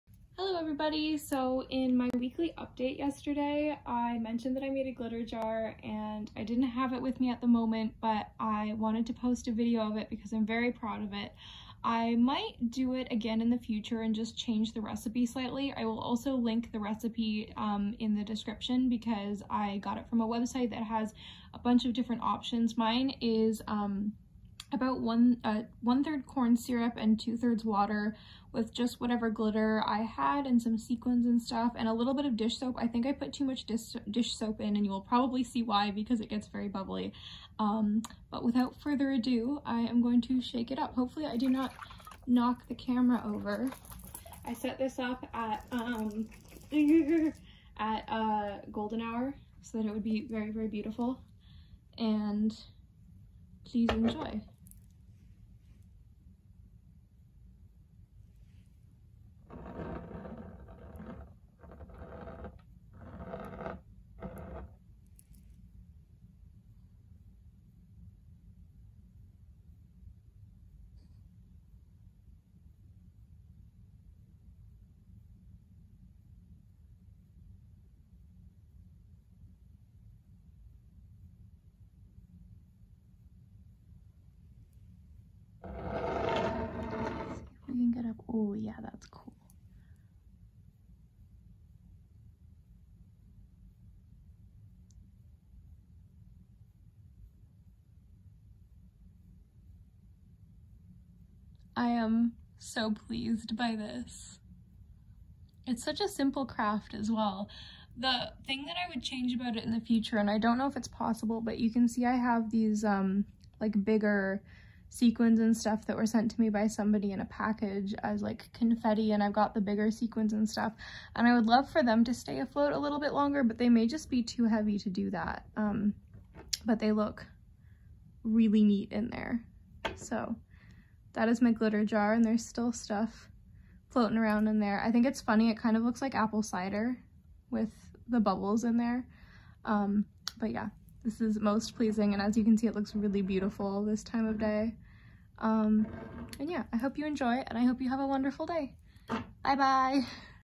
My glitter jar!
My camera did its very best to pick up the glitter but alas it does not look nearly as sparkly as it does in person. I really recommend this as an easy craft that is a great mindfulness tool (especially for kids, I know I made quite a few of these when I was younger). Most of the materials should be things you have around the house and it only takes a few minutes!

I also want to mention that glitter is not very kind on our dear planet, so borrowing it from a friend or finding some second hand is a good idea if you don’t already own some! I have seen “calm down jars” using other materials (I think you could use light plastic beads if you got the consistency of the liquid right!) and I’d love to know if you’ve tried making one yourself!

Info on why glitter is harmful and potential alternatives, should you be curious: